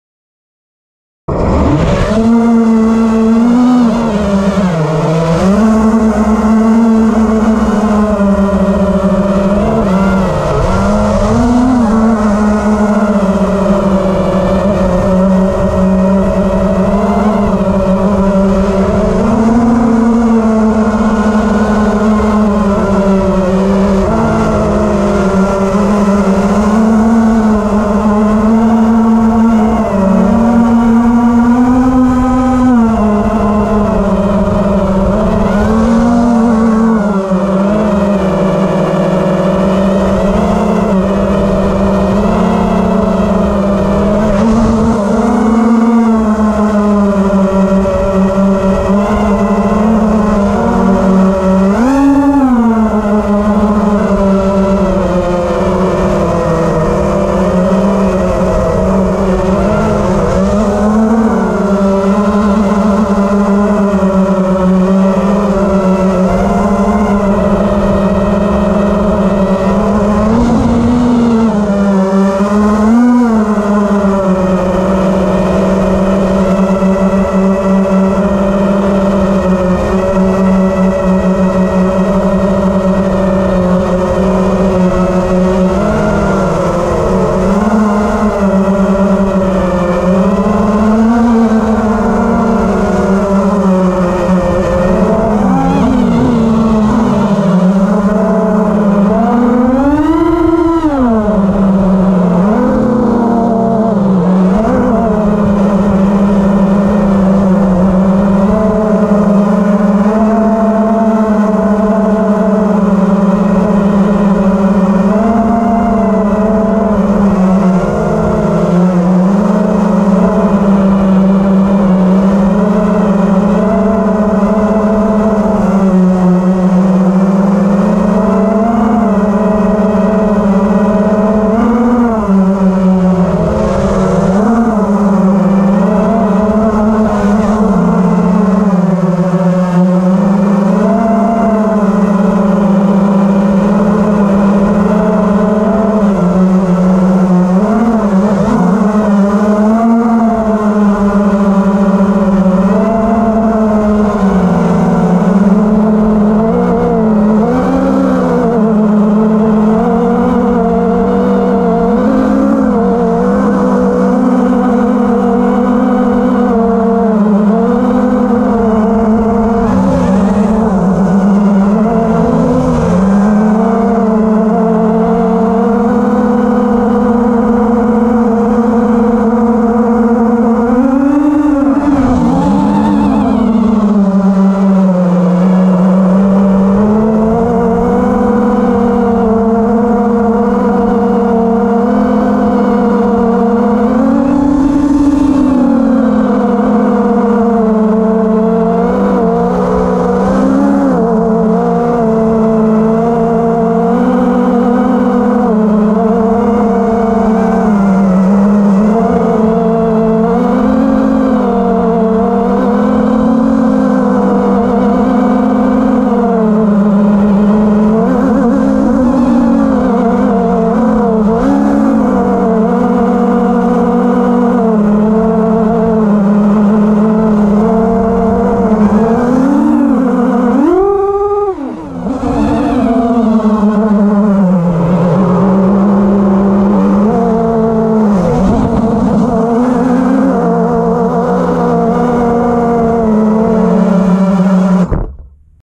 FPV Wizard Quadcopter flying around school playground
Flying my Wizard quad around the field and playground at a local elementary school.